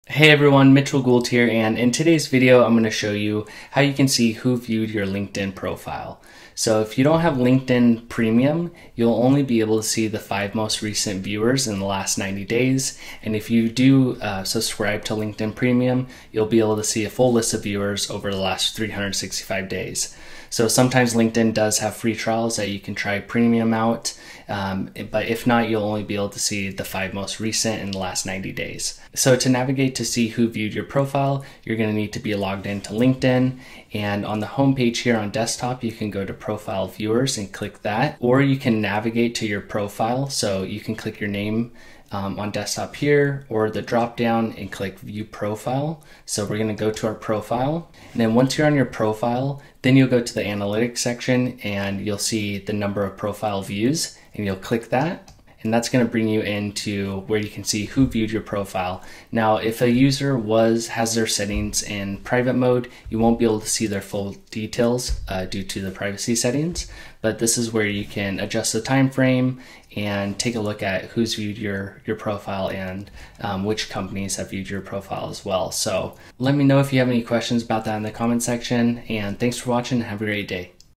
How To See Who Viewed Your LinkedIn Profile (Fast & Easy)
Learn how to see who viewed your LinkedIn profile.

00:00 Introduction
00:06 A note on LinkedIn premium
00:29 Steps to see who viewed your profile
00:59 A note on privacy settings
01:16 Conclusion